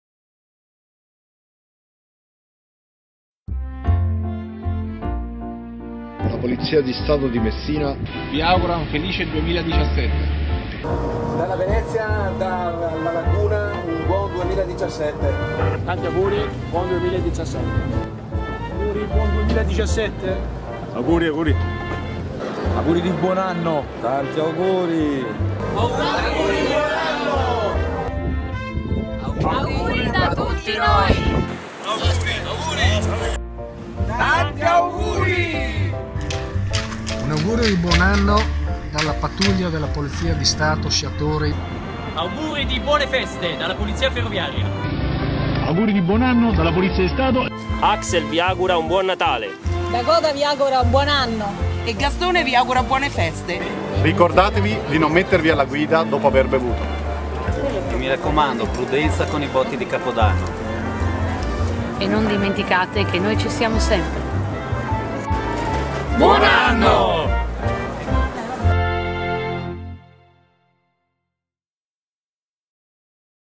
Auguri Capodanno 2017
Gli auguri della Polizia di Stato per il 2017 da parte delle nostre pattuglie e degli specialisti che ininterrottamente anche a Capodanno garantiranno la sicurezza in ogni parte del nostro Paese